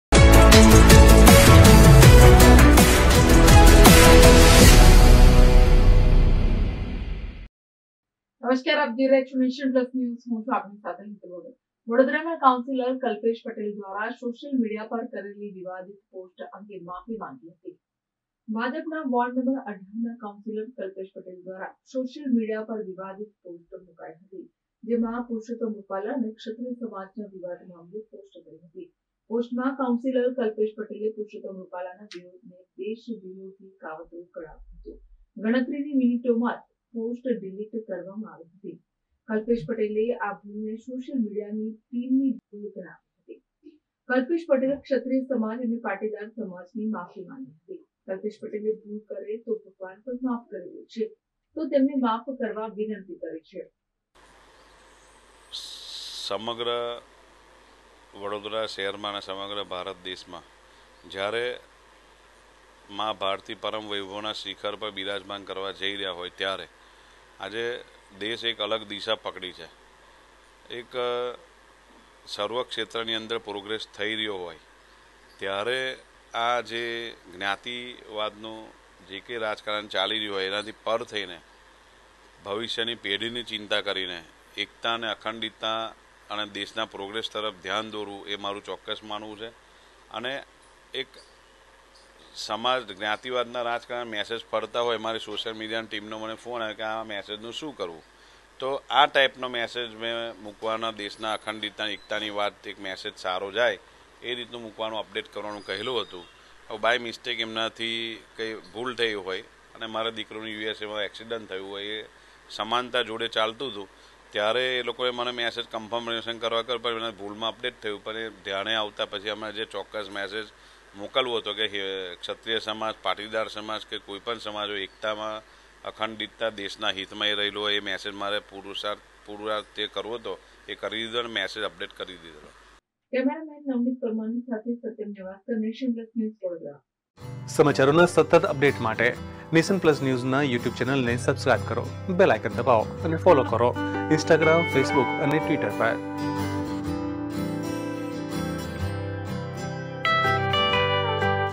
કાઉન્સિલર કલ્પેશ પટેલે કેમ માંગી માફી ?
ભાજપના વોર્ડ ૧૮ના કાઉન્સિલર કલ્પેશ પટેલ દ્વારા સોશિયલ મીડિયા પર વિવાદિત પોસ્ટ મુકાઈ હતી. જેમાં  રૂપાલા અને ક્ષત્રિય સમાજના વિવાદ મામલે પોસ્ટ કરી હતી. પોસ્ટમાં કાઉન્સિલર કલ્પેશ પટેલે પરુશોત્તમ રૂપાલાના વિરોધને દેશ વિરોધી કાવતરું ગણાવ્યું હતું. ગણતરીની મિનિટોમાં જ પોસ્ટ ડીલીટ કરવામાં આવી હતી. કલ્પેશ પટેલે આ ભૂલને સોશિયલ મીડિયા ટીમની ભૂલ ગણાવી હતી. કલ્પેશ પટેલે ક્ષત્રિય સમાજ અને પાટીદાર સમાજની માફી માંગી હતી.